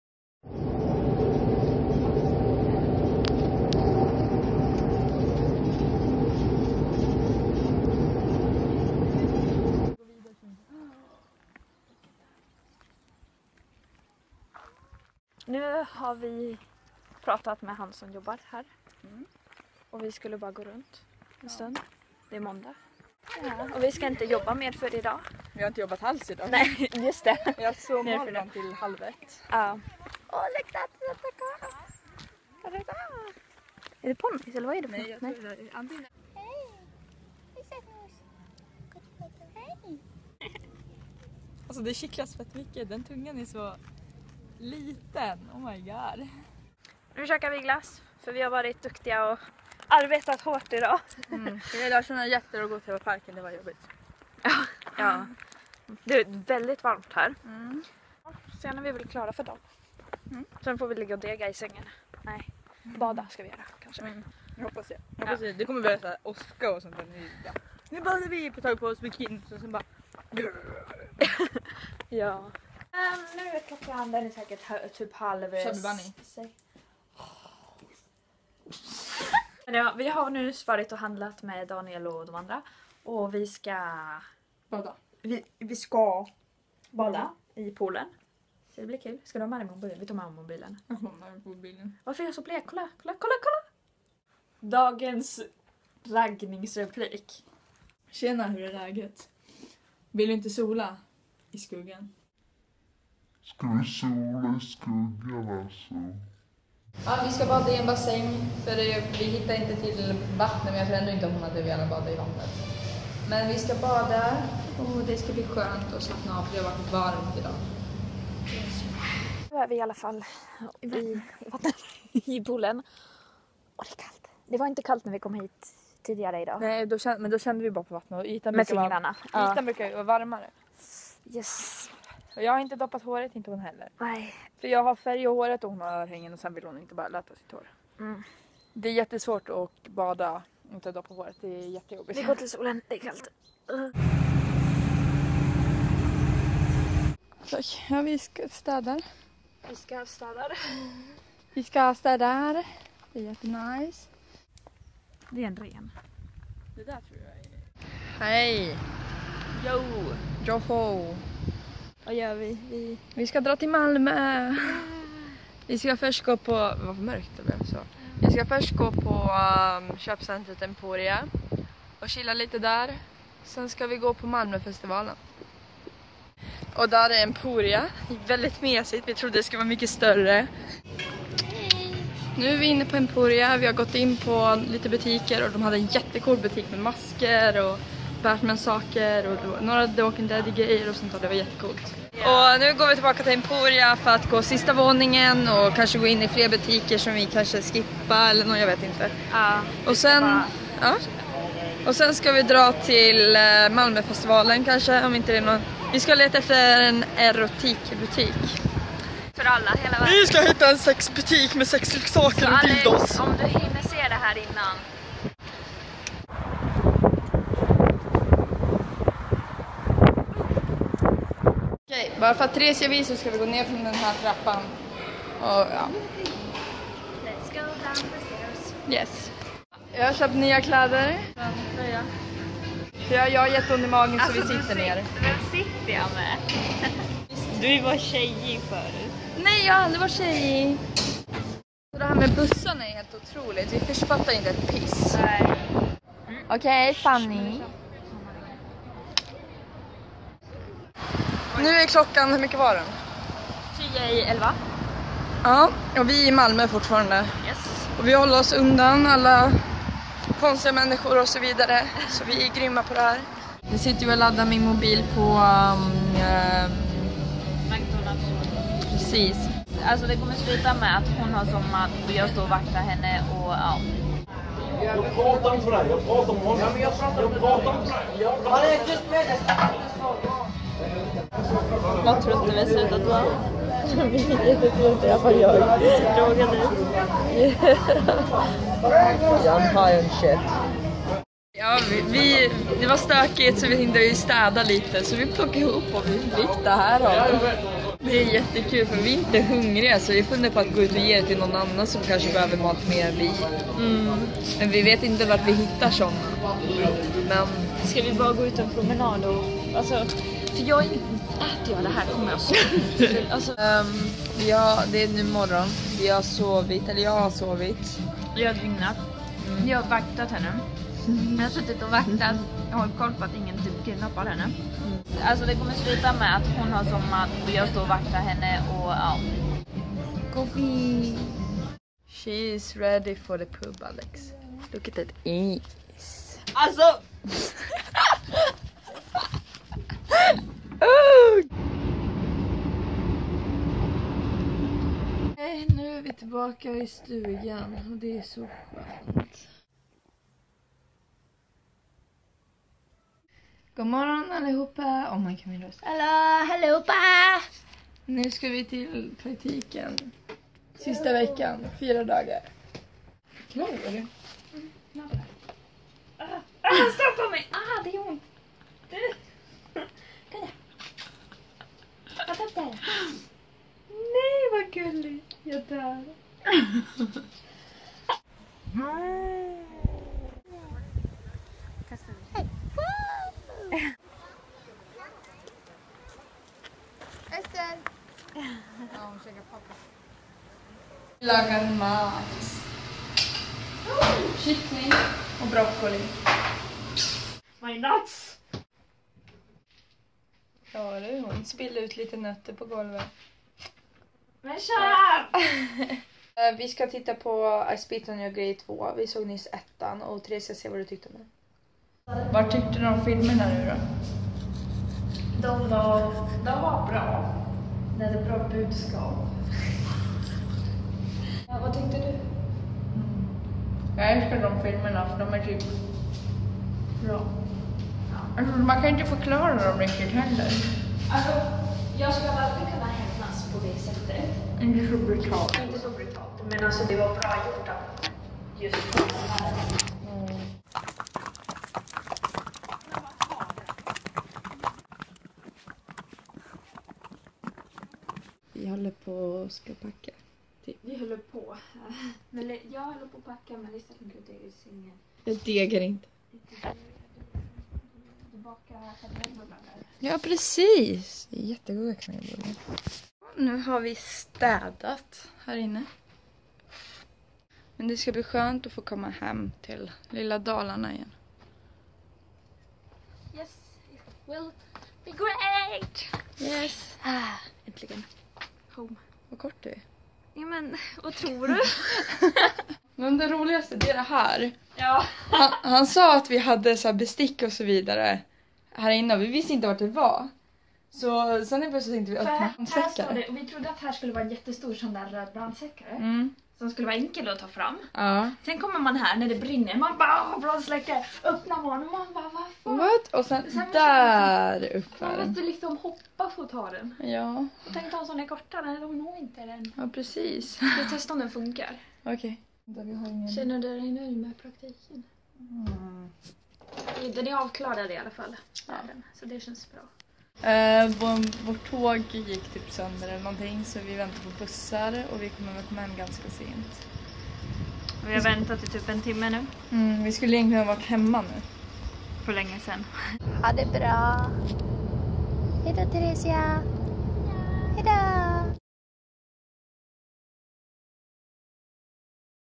Vi drar till Skåne! | Vlog 2#
Vi åkte till Skåne för att ha praktik på skånes djurpark under två veckor!! Här är videoklippet från det!!

Förlåt för dröjsmålet :)